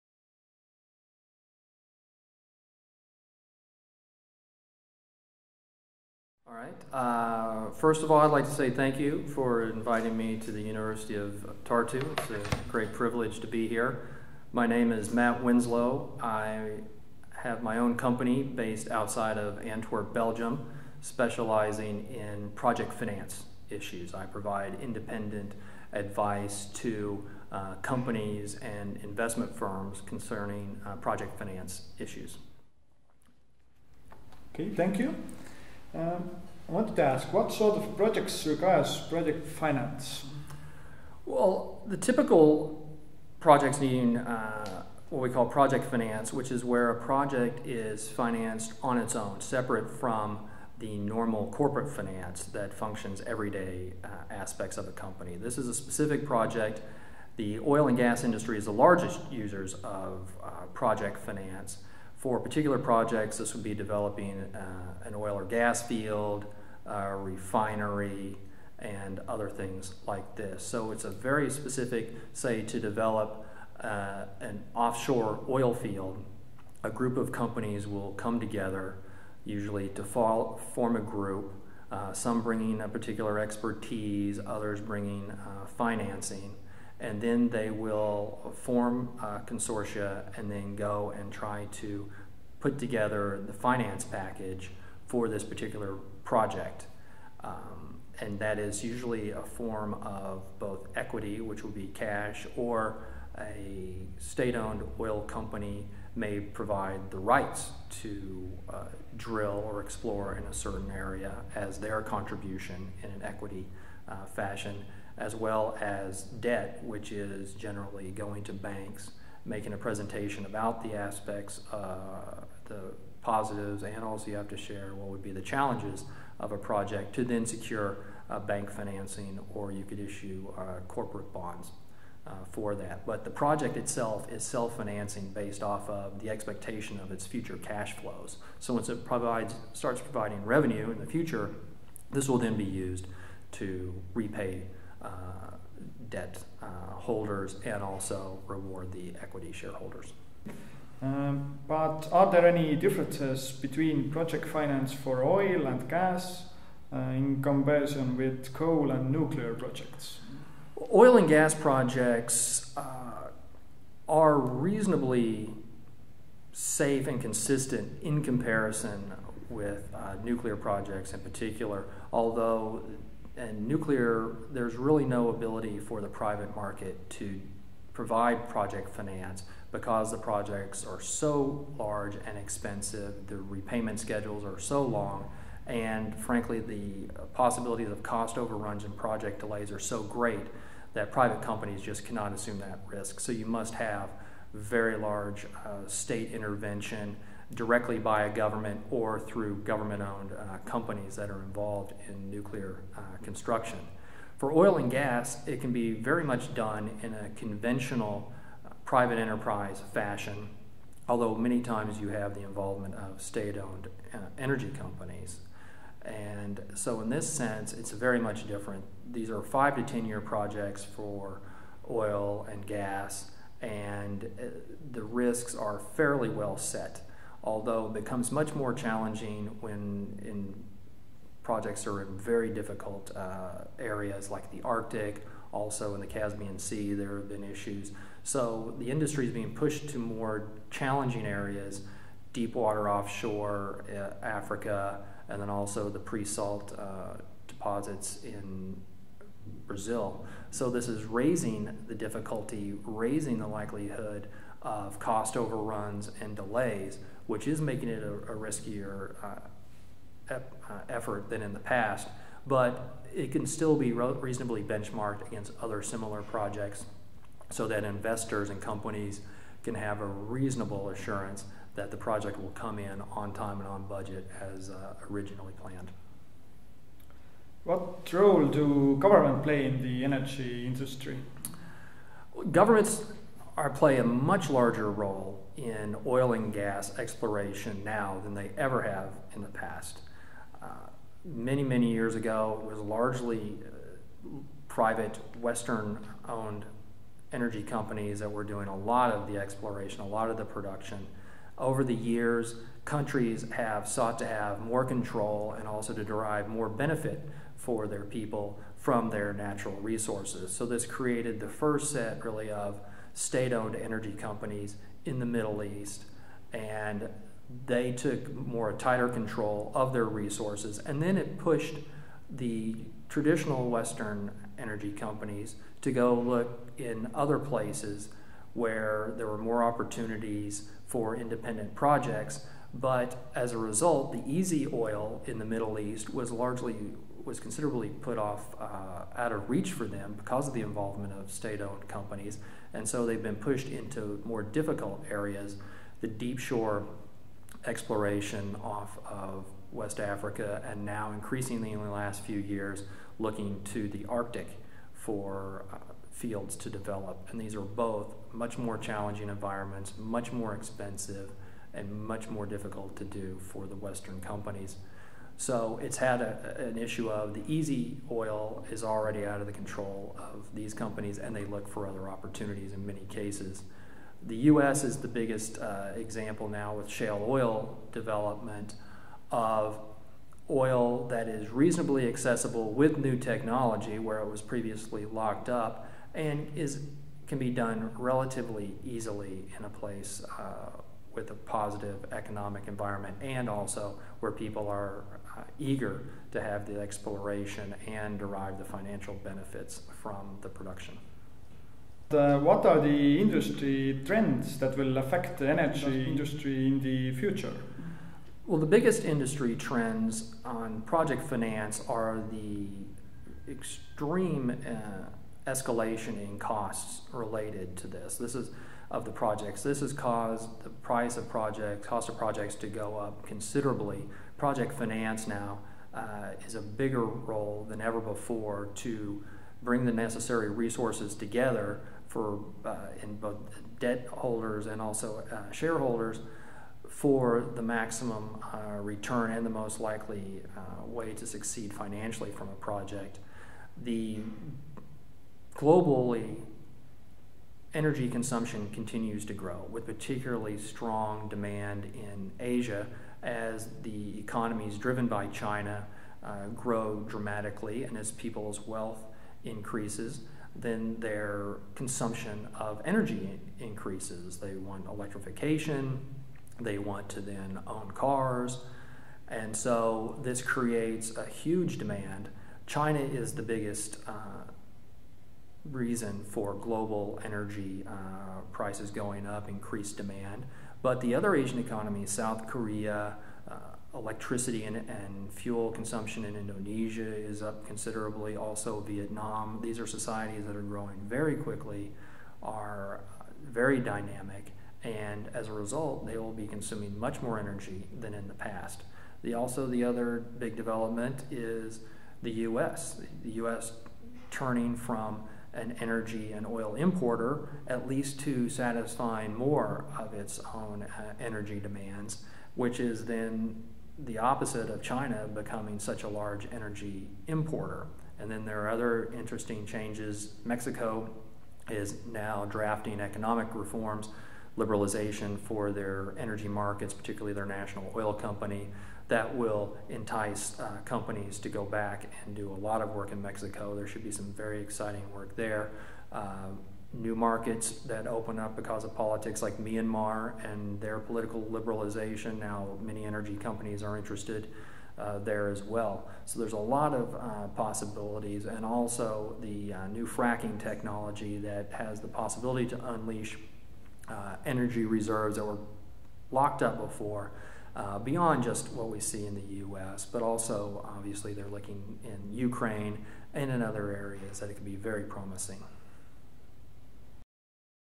Matthew Winslow, Project finance consultant and founder of the Scylax LLC
Interview questions:
1. What sort of projects requires project finance?
2. Are there differences between project finance for oil and gas projects in comparison with coal and nuclear projects?
3. What role do governments play in the energy industry?
4. What are the industry trends that will affect the energy industry in the future?